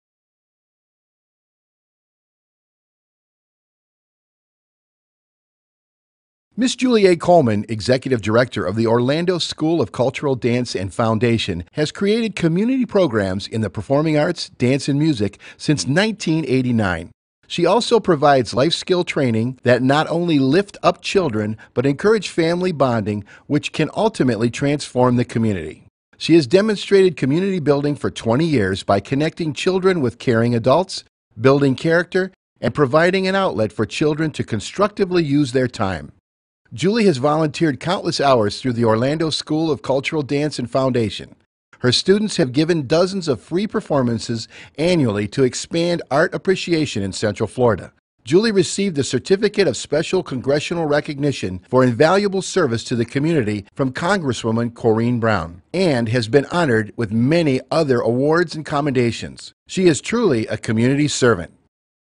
Community Builder 2011
At the 6th annual Orlando Mayor Buddy Dyer's Neighborhood and Community Summit Julie A. Coleman received the 2011 City of Orlando Community Builder Award.